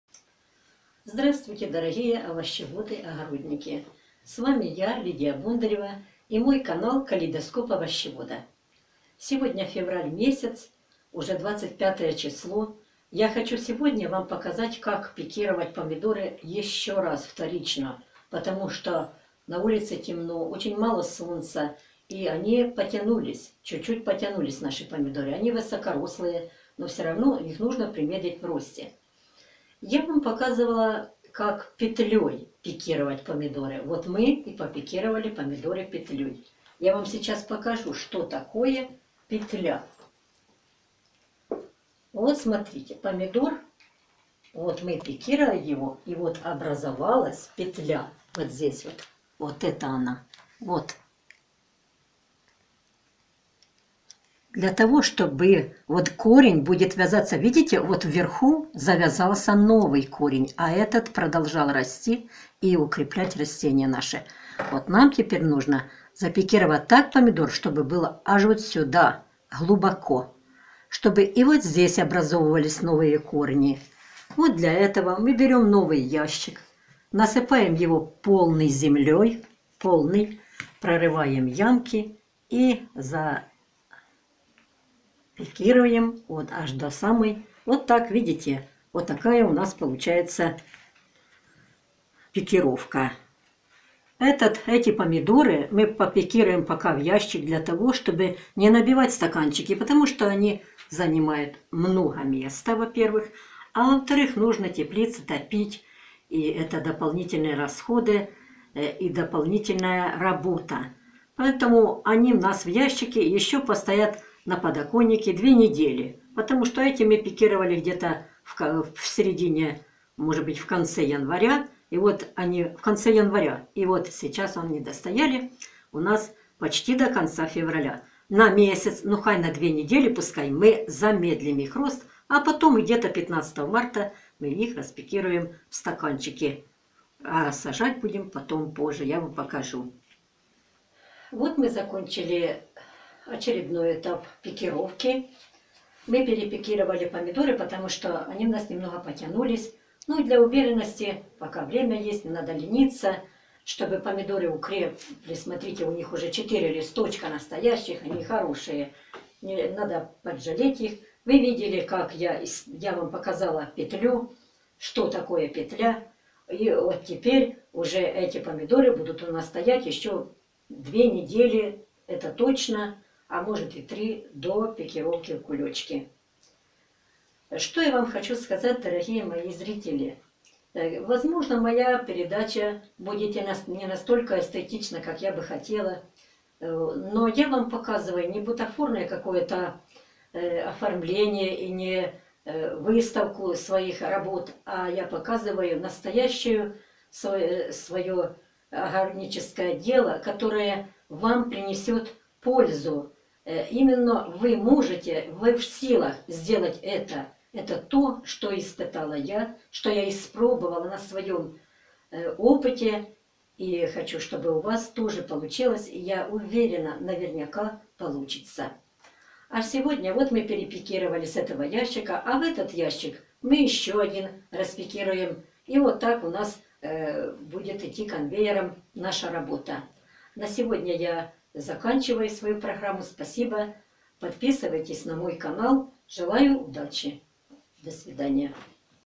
Вторая пикировка петлей
Вторая пикировка петлей помидор еще надежнее укрепит растение, создаст мощную корневую систему и также улучшит качество рассады в целом.